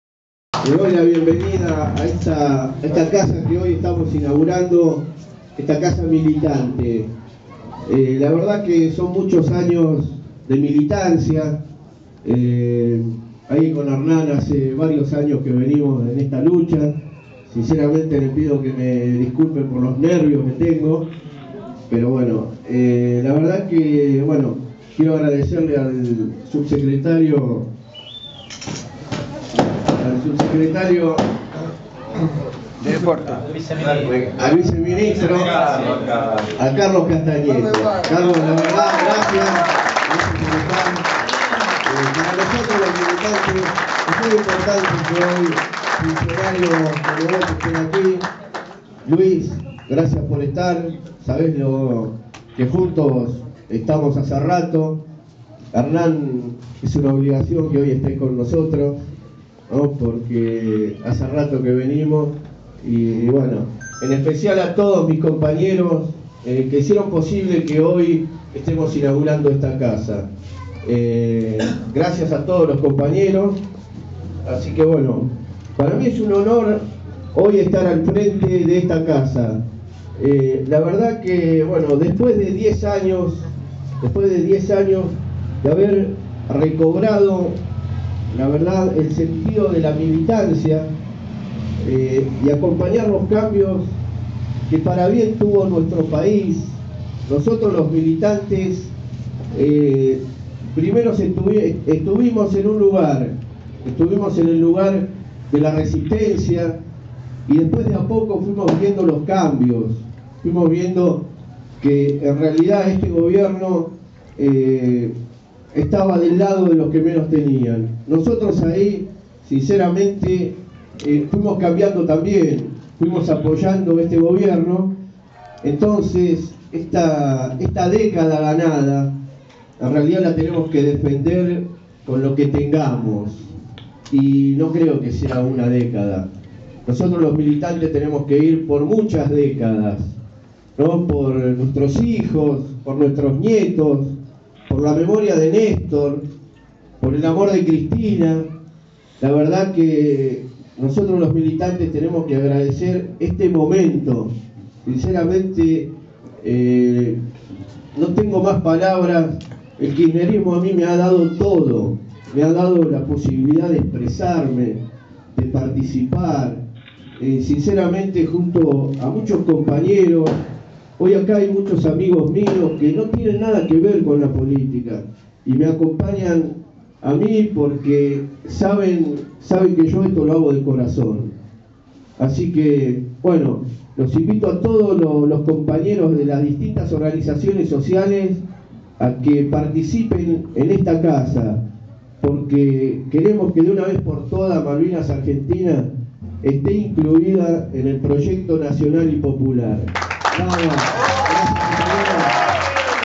Segundo Centenario inauguró su local en Pablo Nogues (Marcelo Caravagio)
APCS-GBAN. Inauguración del local de Segundo Centenario en la localidad de Pablo Nogues, Partido de Malvinas Argentinas. Participaron Carlos Castagneto(ViceMinistro de Desarrollo Social, Hernán Letcher(Responsable Nacional de Segundo Centenario) y Marcelo Caravagio (Responsable Segundo Centenario de Malvinas Argentinas). Tambien se hicieron presentes Luis Vivona y concejales del Frente para la Victoria